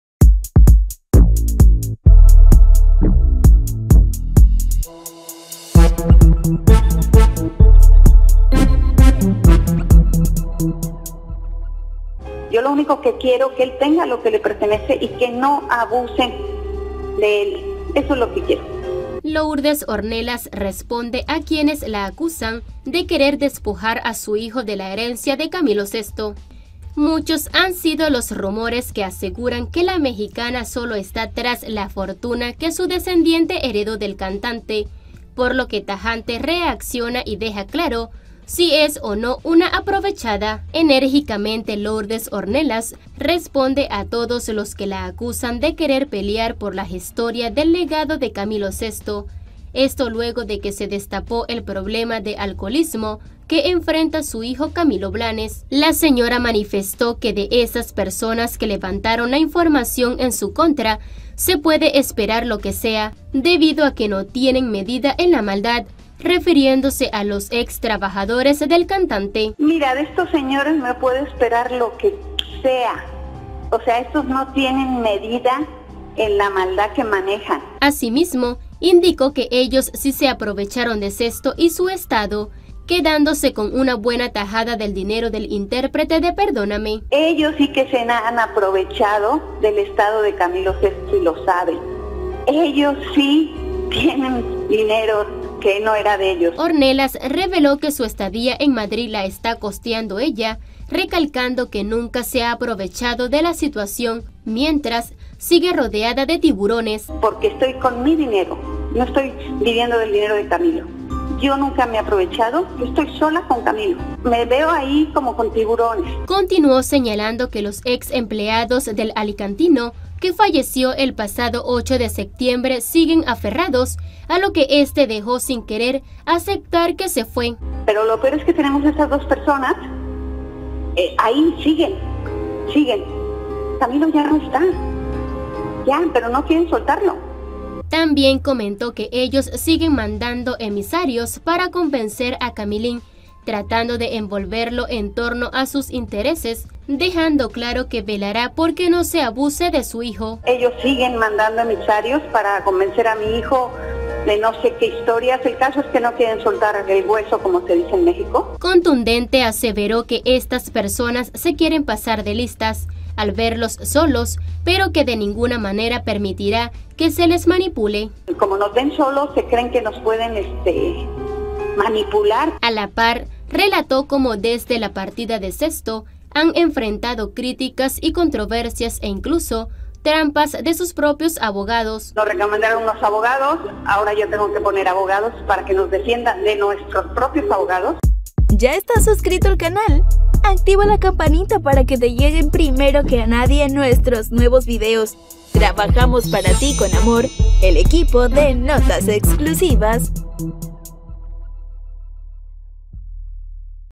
Lourdes Ornelas responde a quienes aseguran que quiere despojar a Camilin de la herencia.
© Image ~
© Song ~
© interprete ~